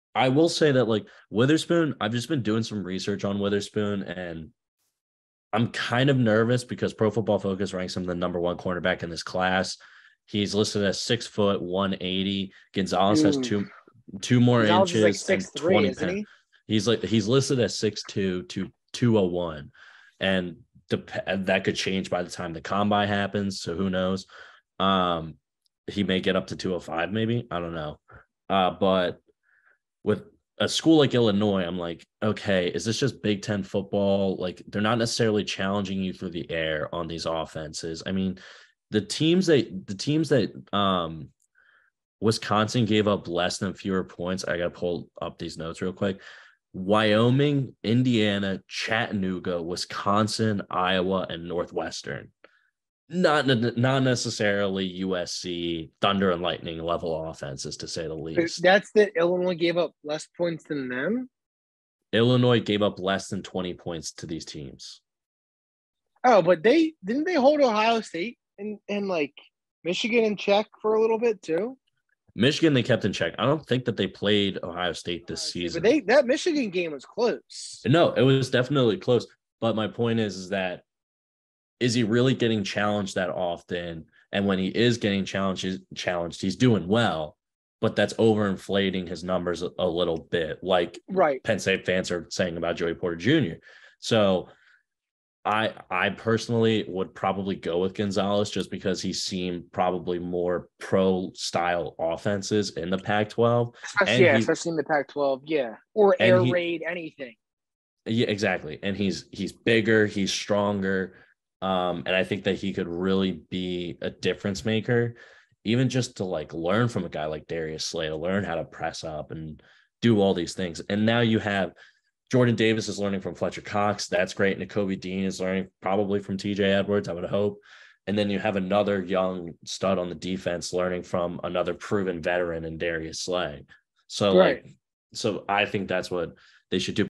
Which cornerback should the Eagles draft in 2023?
The 2023 has three stud cornerbacks: Penn State's Joey Porter Jr., Illinois' Devon Witherspoon, and Oregon's Christian Gonzalez, so the guys debate which corner the Eagles should use a first-round pick on.